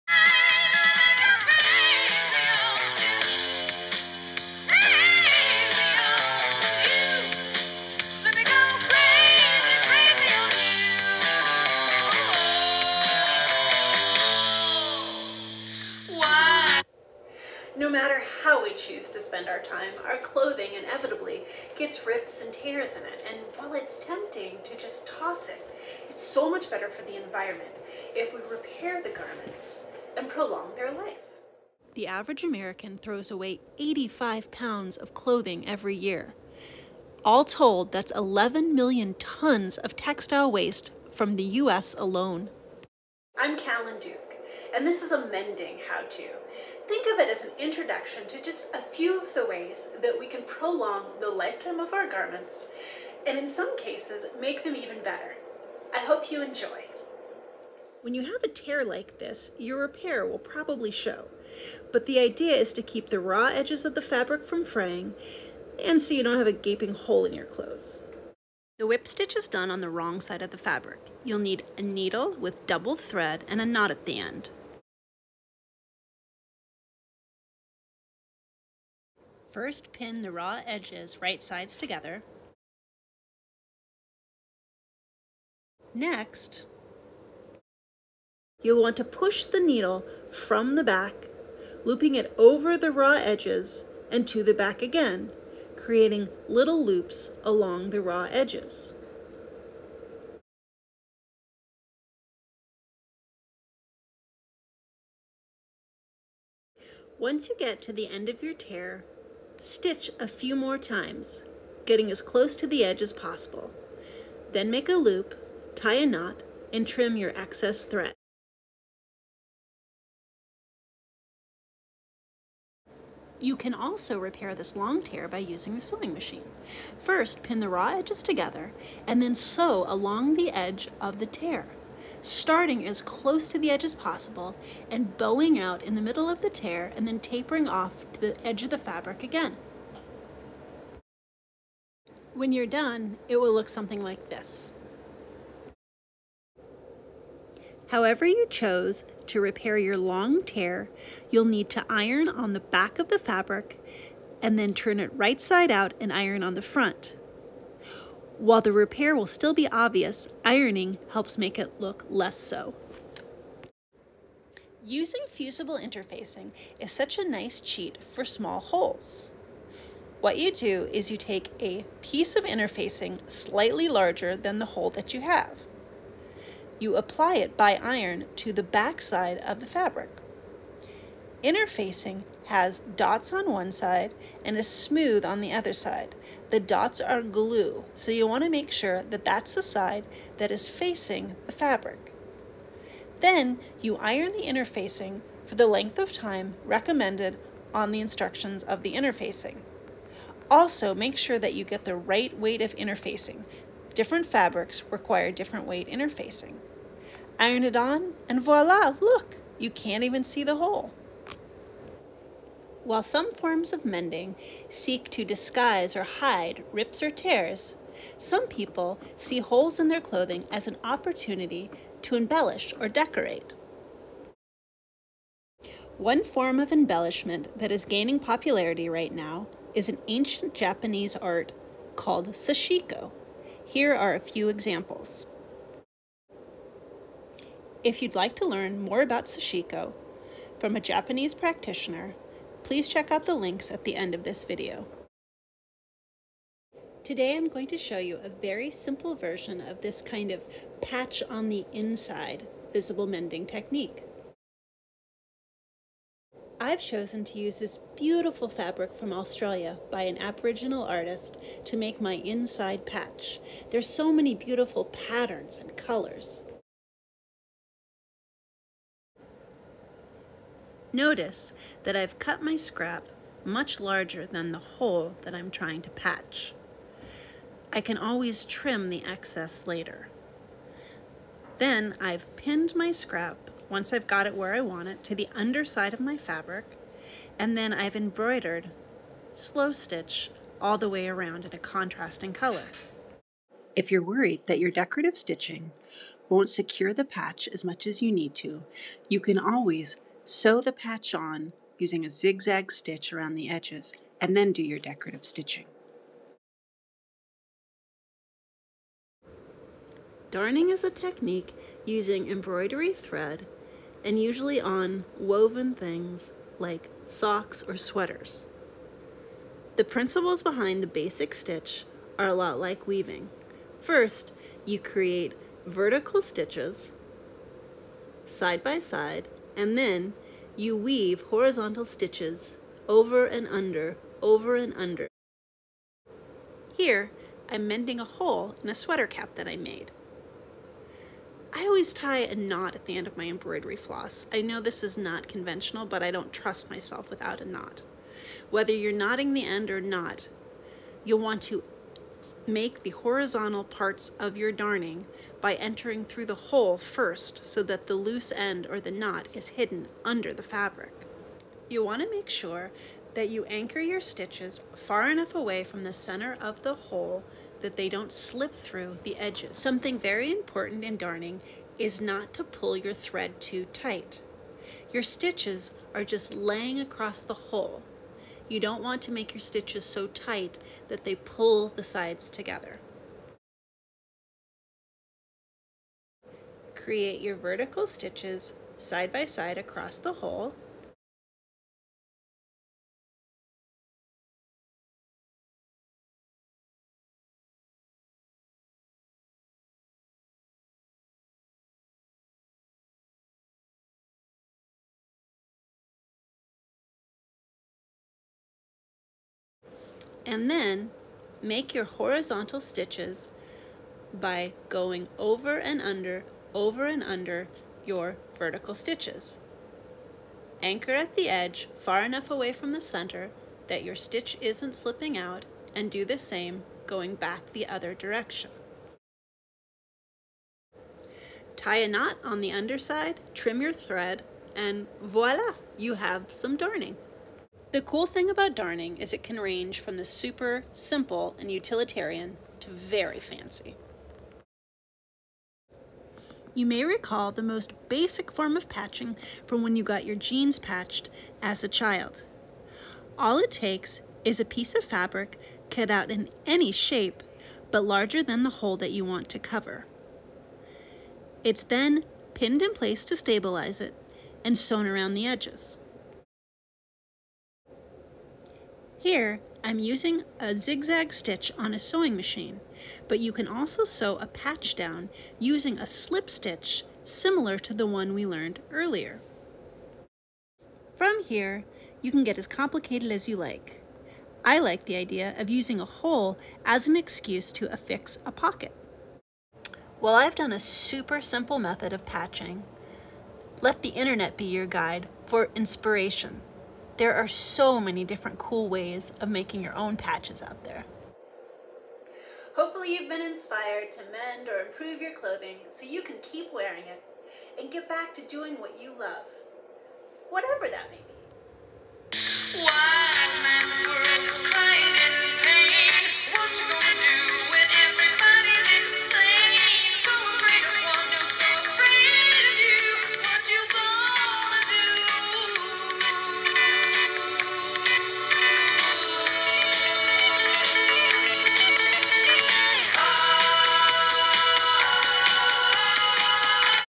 SEWING 101: Mending Holes and Tears
An introduction into some basic mending techniques:
Whip stitch @ 0:59
Interfacing @ 2:43
Visible Mending @ 3:33
Darning @ 5:07
Patching @ 7:25